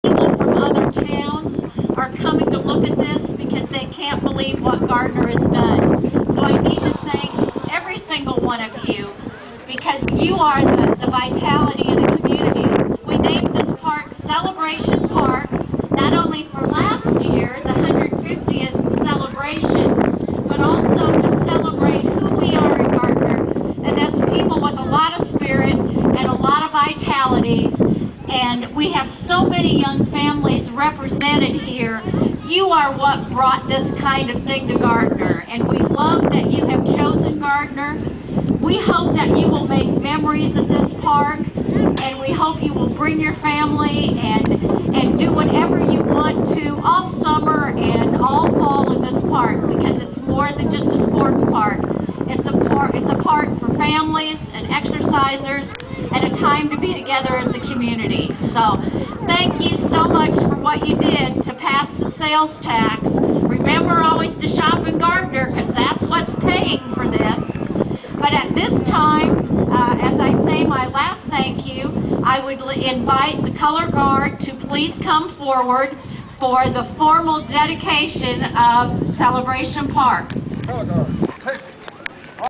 Celebration Park Dedication
Celebration Park in Gardner, Kansas was dedicated on May 10, 2010.